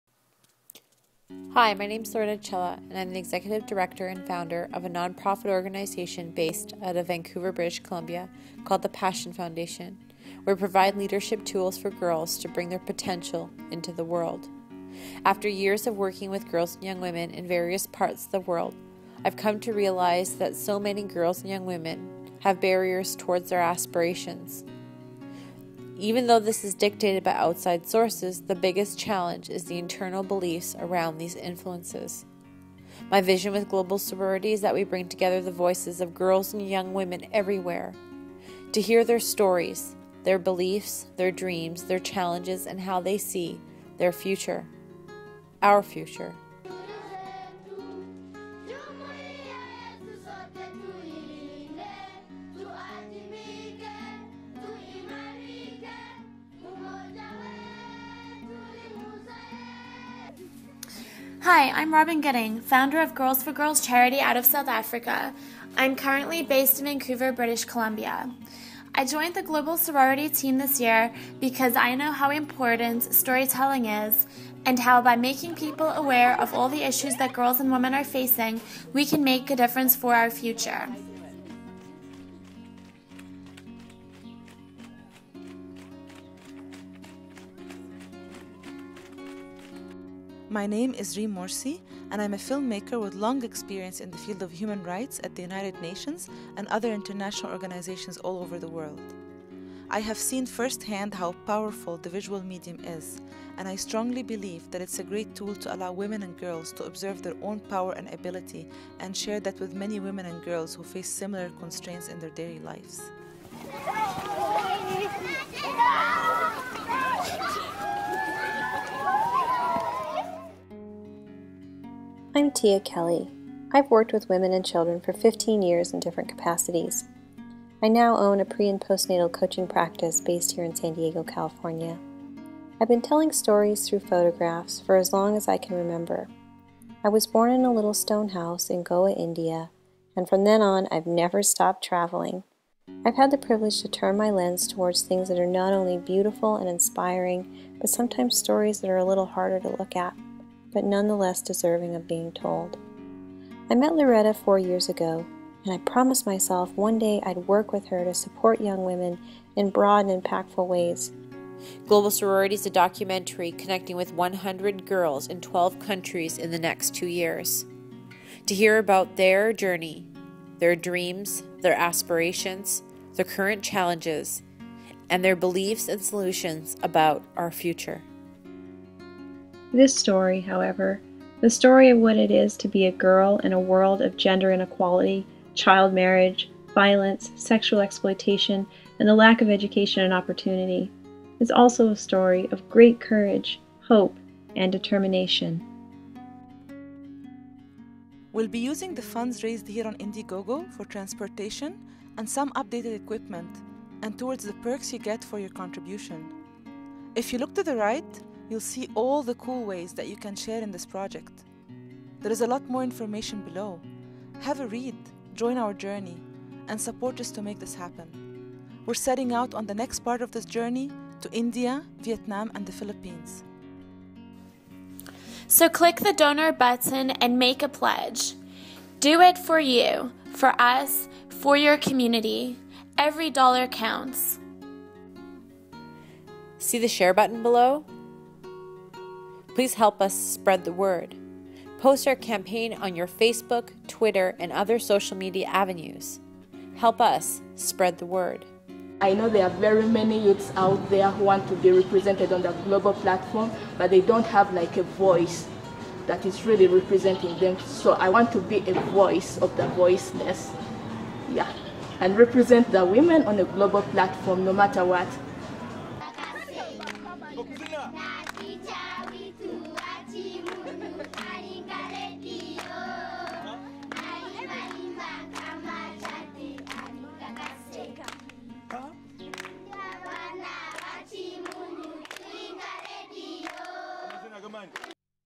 Global Sorority Indiegogo Campaign
How did Global Sorority come to life?
Tia and Loretta, two young women from different parts of North America came together in November 2011 to talk about their work and passion for making the world a better place through focusing on gender specific solutions to global issues.

Through their determination and collaboration, they set out to share the stories of 100 young women from around the world and this is how Global Sorority was born.

What have we done so far?

We filmed the first documentary in this series on a shoestring budget, lots of personal collateral and an incredible amount of community support. The end result was more than worth our time and effort, but without that support from our generous backers it wouldn't have been possible. With that amazing experience behind us, we now know exactly what it takes to produce even a short film of this intense nature. We've received an inspiring amount of interest in our project and it's mission, from opportunities to speak about our journey, to premiering the first Global Sorority series at the Projecting Change Film Festival in April 2013.

To be able to share this next set of stories with the world, we need you to make it happen.

Who's on our team?
Loretta and Tia are excited to have added two amazing women to this project including award winning filmmaker Reem Morsi and Girls4Girls founder Robyn Gooding! We are now four women from four parts of the world coming together to make change happen.

Together with your support of this campaign we strive to create a truly beautiful and inspiring film series.

For more about us and our work, you can visit us on the web.

With your support we can create the next documentary in this important series. Be a part of the global shift towards peace and stability through the proven method of supporting women's equality and education world wide.

We are committed to doing more than just making a documentary!
Along with our larger goal of creating a film series that supports awareness and global community around gender issues, we are also providing empowerment and leadership workshops for the young women who share their stories with us.

Instead of just leaving with something from them, we are providing tools that will assist these young women for life. We believe that it's important to leave each girl we connect with, tools to support them in making an internal shift; showing them that they are important. We know all the education and support from outside sources won't guarantee a girl will realize her full potential. She must first believe in herself.

By contributing to this film in any way you can, you will be creating awareness and supporting the idea that girls everywhere deserve to be valued, educated and equal and that this will be the catalyst to a brighter future for us all.

Here's a break down for you in more detail:
Where is your money going?

We will be using all of the funds from this campaign on transportation to and around the countries visited, budget accommodations, updating some filming equipment, and  towards your backer rewards. we are applying for outside grants for additional costs (such as post production) to complete the documentary. We are donating the leadership training and materials for the girls and each of us will take the time off from our "other jobs" to create this film and will not receive a salary from this campaign.
The Impact
By giving to this project you aren't just funding a documentary and providing leadership tools to girls in Indian, Vietnam, and the Philippines you are also supporting us in creating an educational series that will be used in schools and communities in North America to start a proactive discussion on how we can all come together to support each other and our world.
Other Ways You Can Help

Perhaps you can't help because money is tight right now (we know it's not always possible) so there are other ways we'd love your help: You can help generate traffic to this campaign by adding our story to your blog. You can click the share button and help spread the word in your social media networks and within your community. You can share the link within your company and get a team donation going. Maybe your birthday is coming up and instead of gifts you can direct people to our campaign to make a donation in your honour. Or maybe you have some time and could do a small fundraiser. Thank you to all our present and past supporters for joining our "virtual" team.